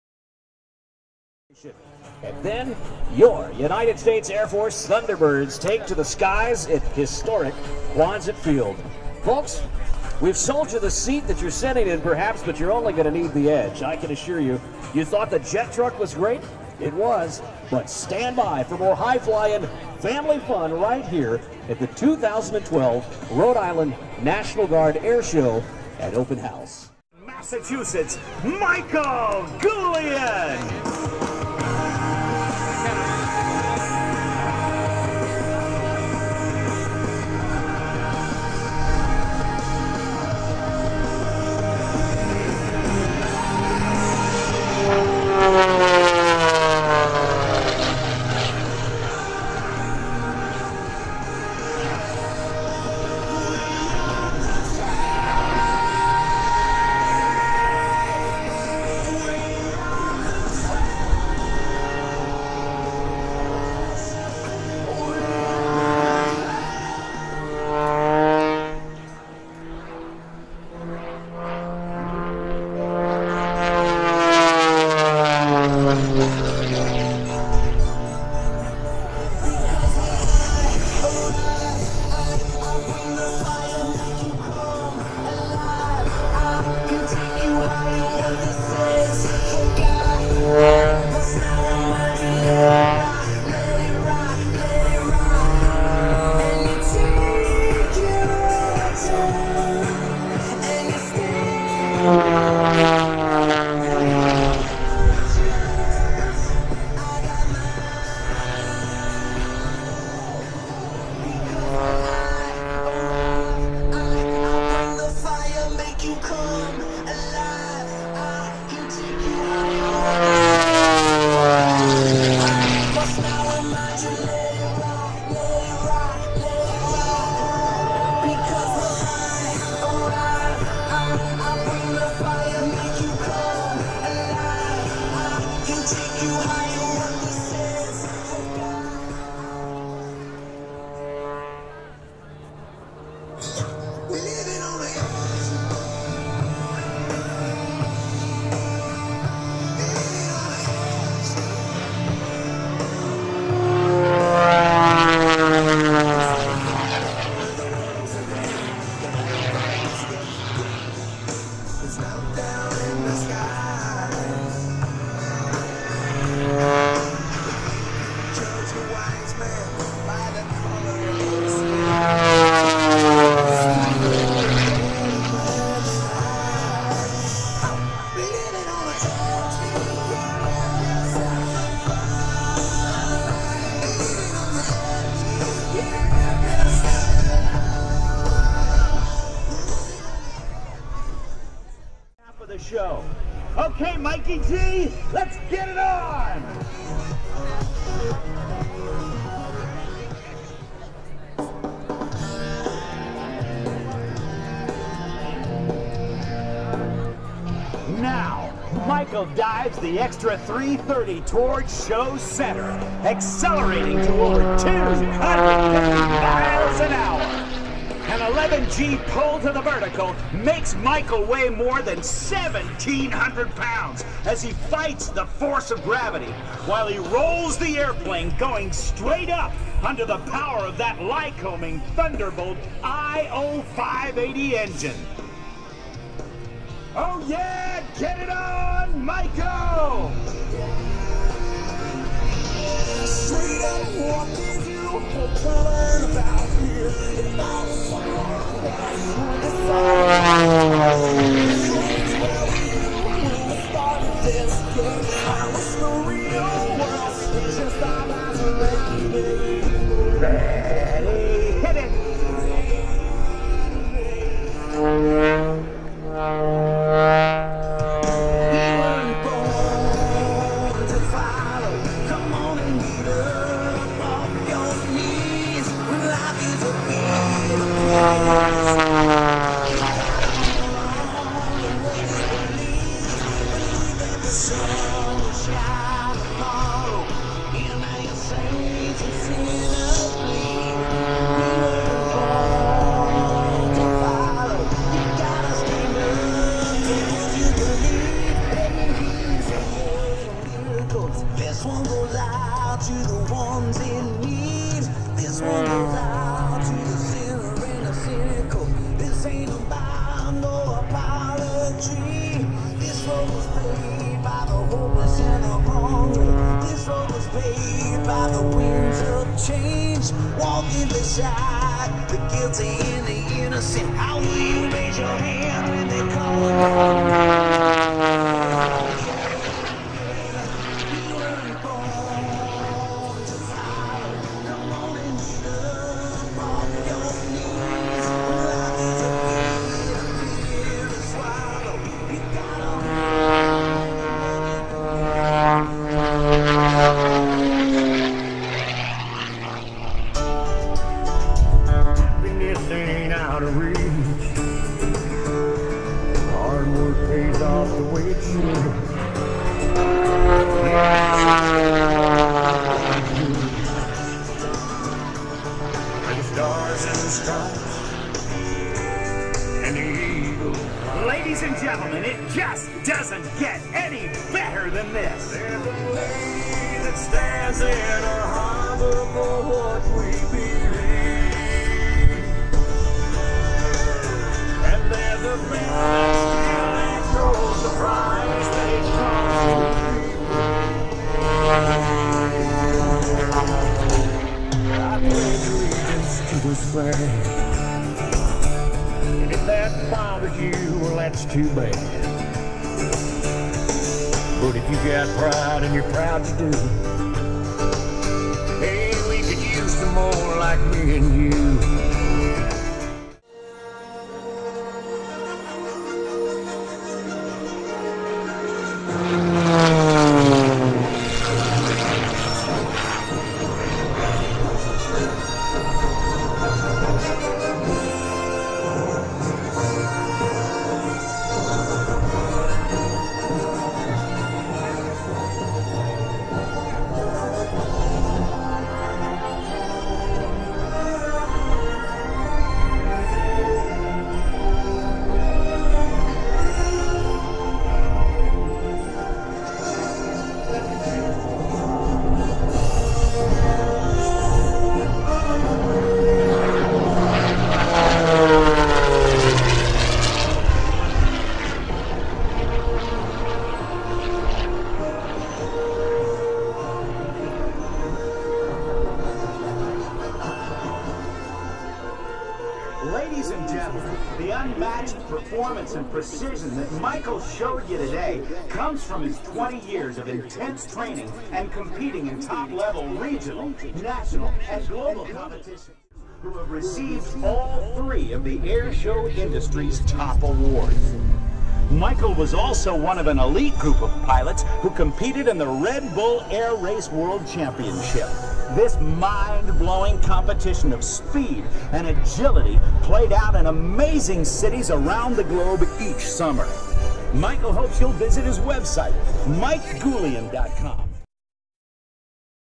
2012 R.I. Airshow at Quonset - Michael Goulian in the Extra 330
Rhode Island National Guard Open House & Air Show.

Michael Goulian in the Goodyear Sponsored Extra 330 and his full afternoon aerobatic flight performance.

Location:     Quonset Airport (Quonset Air National Guard Base)
City/Town:   North Kingstown, R.I.
Date:           Sunday 17th, 2012